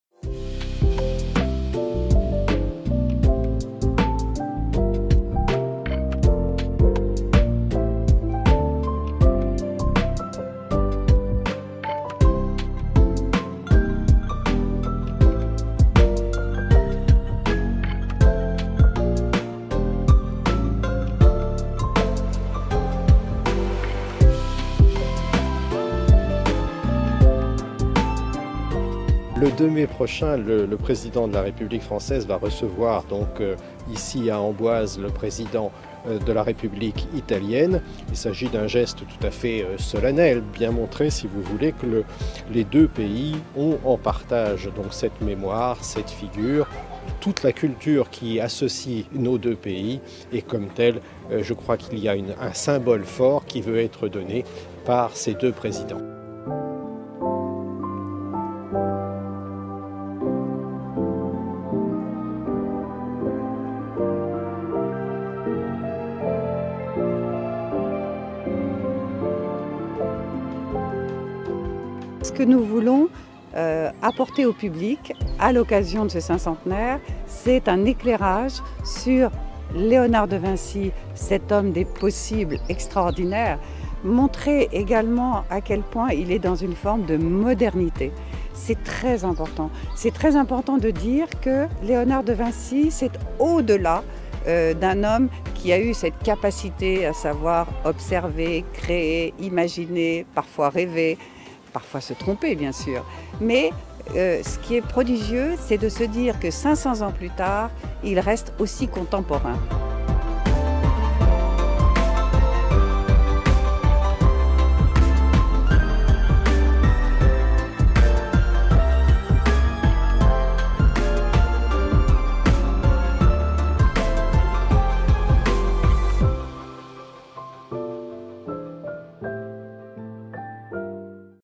At Amboise, Leonardo's last years paint a picture of Franco-Italian harmony
Commemorations for Leonardo da Vinci's 500th anniversary begin this week in Amboise, in the Loire Valley, with France and Italy setting aside recent tensions to honor the memory of the Renaissance genius in the town where he spent his final years.
In 1516, aged 64, Leonardo da Vinci left Italy to enter the service of King Francis I of France. Many of his masterpieces - St. John the Baptist, the Mona Lisa - followed him and were sold to the French monarch, forming a legacy now exhibited at the Louvre museum in Paris.
Amid diplomatic tensions between Rome and Paris, his legacy has become contentious, with Italy's Culture undersecretary Lucia Borgonzoni in November telling Italian media she wanted to renegotiate the planned lending of his works to the Louvre for an anniversary exhibition, because "the French cannot have it all."
It is unclear, for example, whether the iconic drawing of the "Vitruvian Man" will eventually leave Venice to join the Louvre for the display.
But on Thursday (May 2), in Amboise, French President Emmanuel Macron and Italian counterpart Sergio Mattarella will seek to ease strains between the two normally close allies that have grown more acute since mid-2018, mostly over migration policy.
They will gather at Leonardo's tomb, a modest grave in a chapel of Amboise castle containing his presumed remains, and will pay a visit to his house nearby, the Clos Luce, where he died on May 2nd, 1519.
Da Vinci 's arrival in France was no accident, because King Francis I wanted him to join the Court to participate in its international influence and refinement, Sureau said.
"Leonardo da Vinci was unquestionably born in Italy, he's Florentine, but beyond that, he led a career at the service of several powerful men. This career, and his life, end here, in France," Sureau added.
During his three years in France, da Vinci focused on perfecting unfinished masterpieces, drawing and scientific writing, but also took part in organizing lavish parties for the King of France.